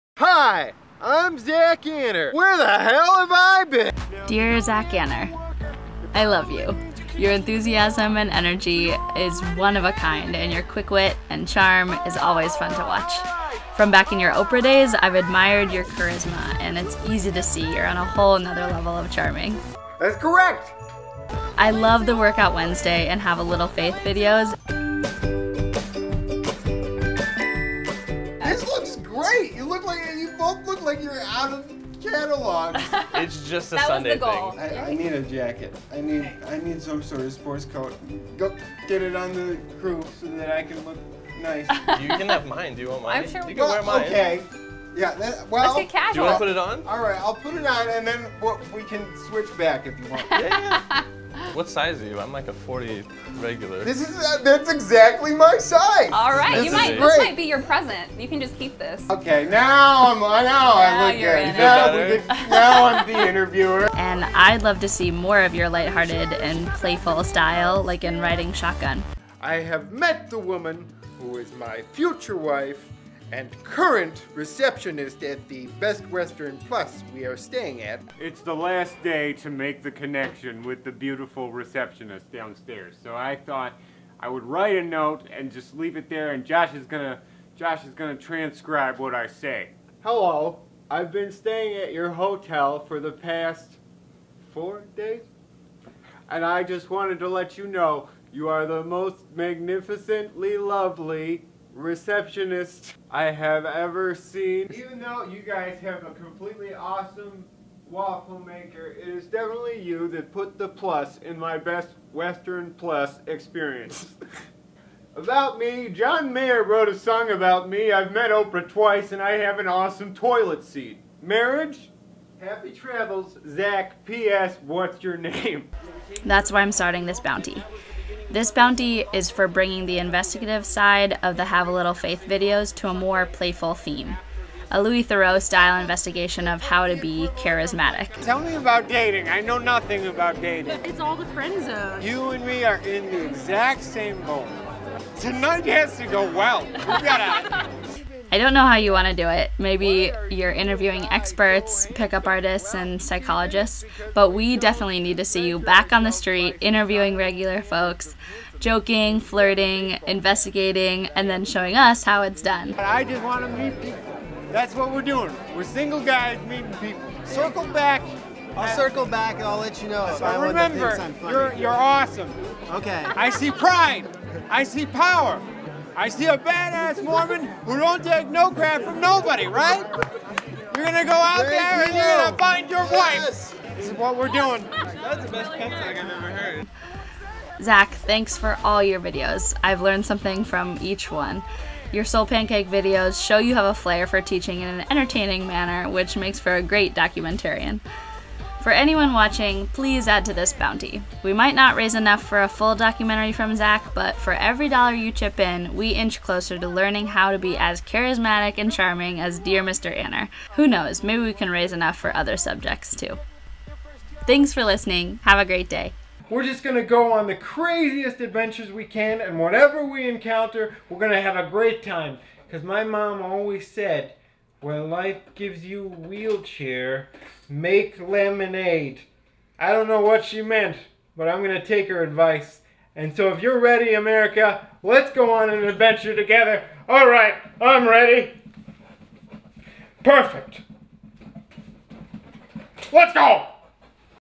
Dear Zach Anner, charisma documentary?
We're starting a bounty @ for Zach Anner to make a documentary on charisma! We're aiming for over $1000 in the hopes we'll get his attention and see something made!

Suggested rewards include bloopers, producer credits, google hangout, and any other ideas you might have.